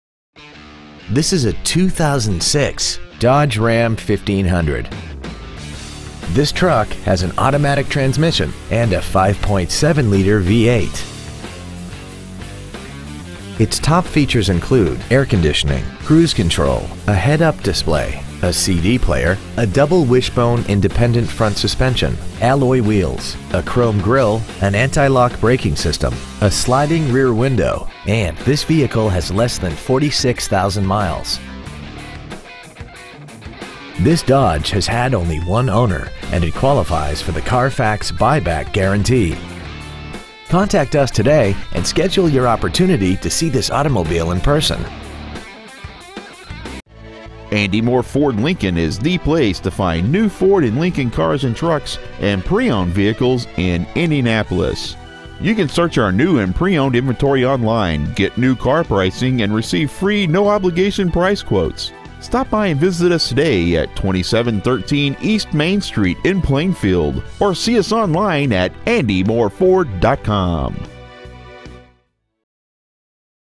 2006 Dodge Ram 1500 Indianapolis IN 46168
Year : 2006
 Make : Dodge
 Model : Ram 1500
 Engine : 5.7L V8
 Trans . : AUTO W/OD
 Exterior : Gold
 Miles : 45,449
 Interior : Khaki
 Stock : P6442

 2713 E. Main Street